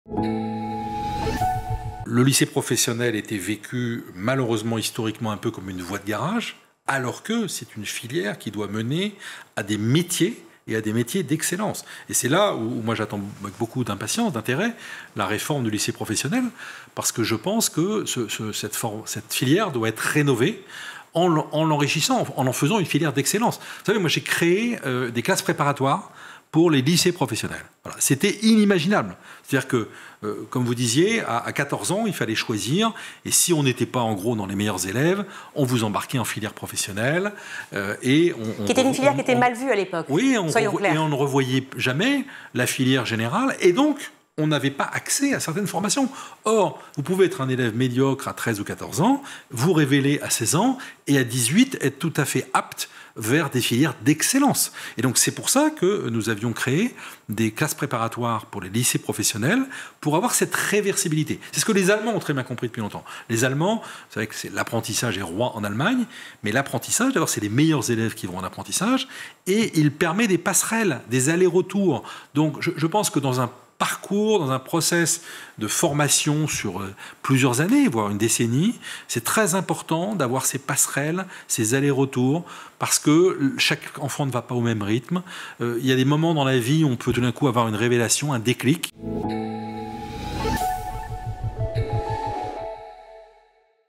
Voie professionnelle : un gage de réussite, selon Luc Chatel
En Europe, 48,7% des élèves de second cycle de l'enseignement secondaire suivent un enseignement professionnel, contre 39,3% en France pour l'année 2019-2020, selon une étude Eurostat. Pour Luc Chatel, ancien ministre de l'Éducation nationale, la filière pro "doit mener les élèves à des métiers d'excellence". Il déplore cependant le manque de passerelles entre les formations et insiste sur l'importance de rénover la filière en l'enrichissant, notamment grâce à la réforme de la voie professionnelle.